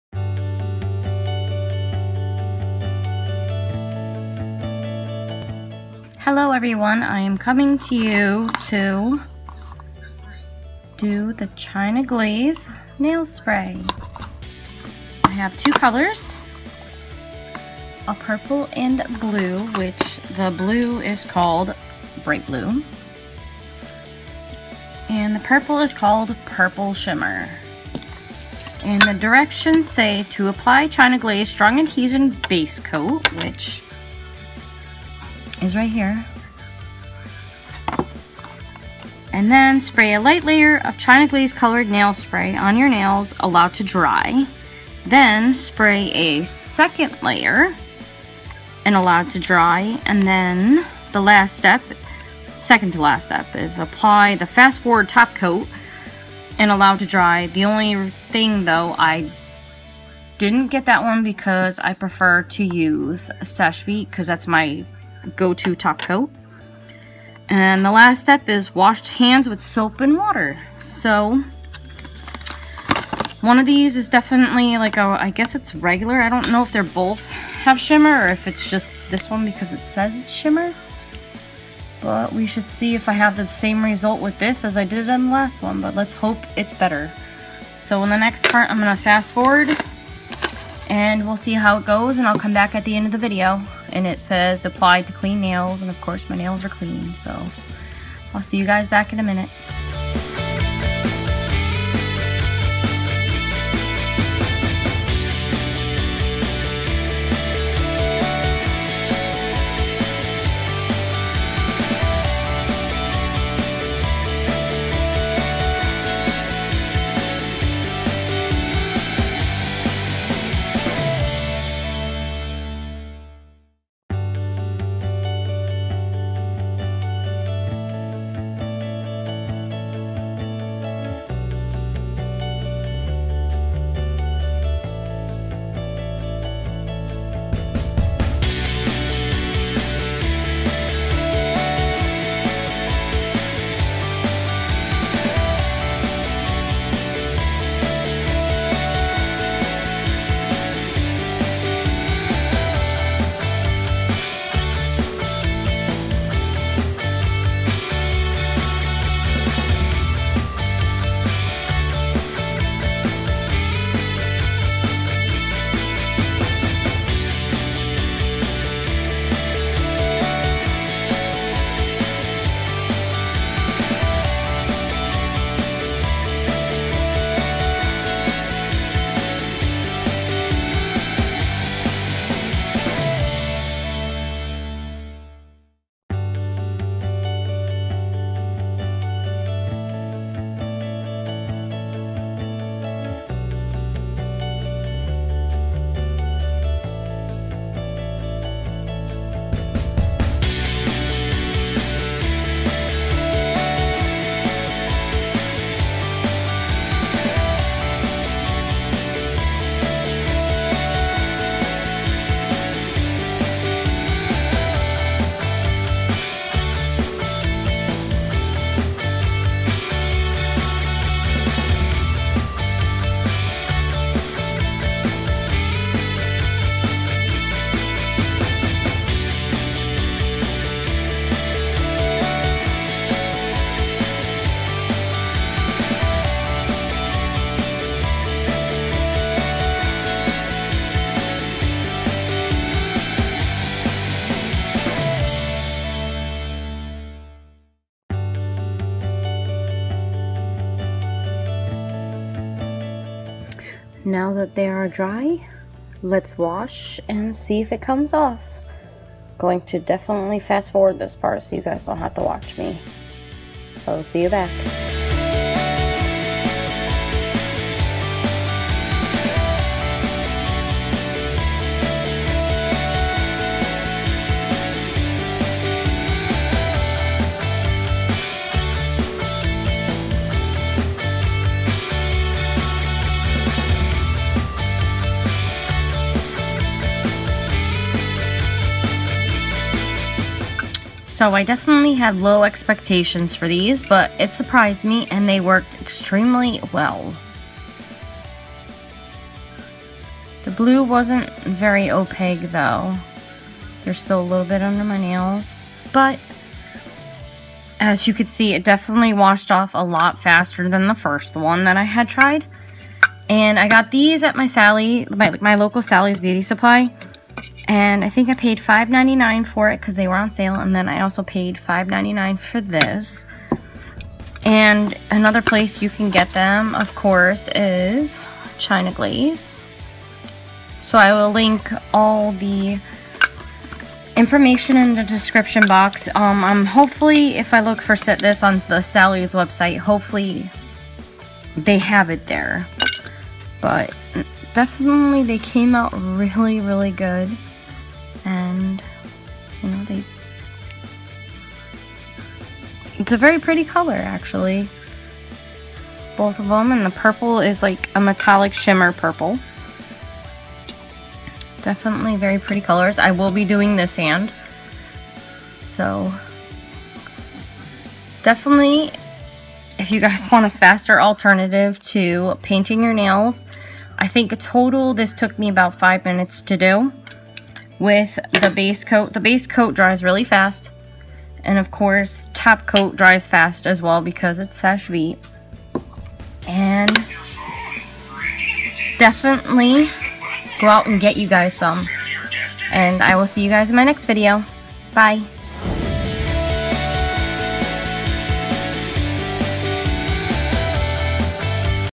China Glaze Nail Spray!! Does This WORK????
I WAS SO SHOCKED THAT THIS PRODUCT WORKED I WAS VERY IMPRESSED WITH IT, IF U WOULD LIKE TO PURCHASE IT U CAN FIND IT HERE: